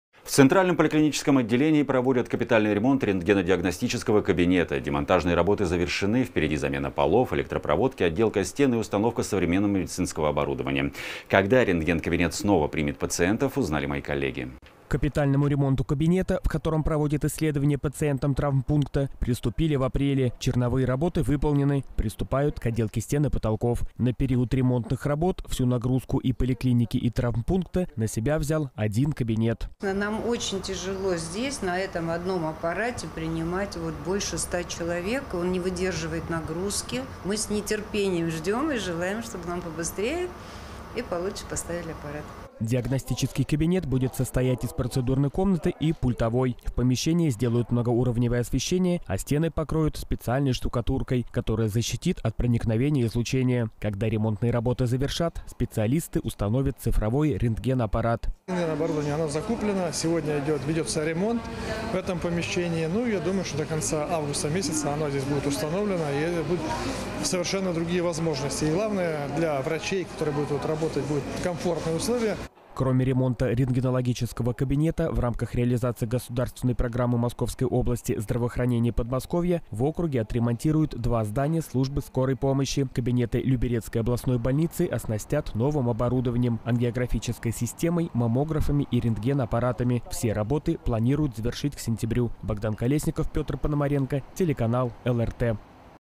В поликлинике у «Вечного огня» установят цифровой рентген-аппарат
В Центральном поликлиническом отделении проводят капитальный ремонт рентгенодиагностического кабинета. Демонтажные работы завершены, впереди – замена полов, электропроводки, отделка стен и установка современного медицинского оборудования. Когда рентген-кабинет снова примет пациентов, узнали мои коллеги.

К капитальному ремонту кабинета, в котором проводят исследования пациентам травмпункта, приступили в апреле. Черновые работы выполнены. Рабочие приступают к отделке стен и потолков. На период ремонтных работ всю нагрузку и поликлиники, и травмпункта на себя взял один кабинет.

Любовь Корнилова, врач-рентгенолог Центрального поликлинического отделения:

«Нам очень тяжело здесь, на этом одном аппарате, принимать больше 100 человек. Он не выдерживает нагрузки. Мы с нетерпением ждём и желаем, чтобы нам побыстрее и получше поставили аппарат».

Диагностический кабинет будет состоять из процедурной комнаты и пультовой. В помещении сделают многоуровневое освещение, а стены покроют специальной штукатуркой, которая защитит от проникновения излучения. Когда ремонтные работы завершат, специалисты установят цифровой рентген-аппарат.

Владимир Ружицкий, глава г.о. Люберцы:

«Рентген-оборудование уже закуплено. Сегодня ведётся ремонт в этом помещении. Я думаю, что до конца августа оно здесь будет установлено. И будут совершенно другие возможности. И главное, для врачей, которые будут тут работать, будут комфортные условия».

Кроме ремонта рентгенологического кабинета в рамках реализации государственной программы московской области «Здравоохранение Подмосковья» в округе отремонтируют два здания службы скорой помощи. Кабинеты Люберецкой областной больницы оснастят новым оборудованием: ангиографической системой, маммографами и рентген-аппаратами. Все работы планируют завершить к сентябрю.